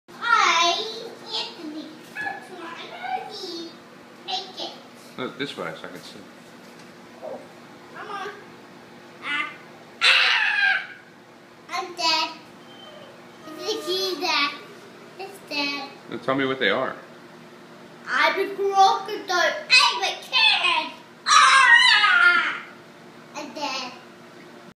Anthony Petruzzi shadow puppetry
Crocodile eats a kid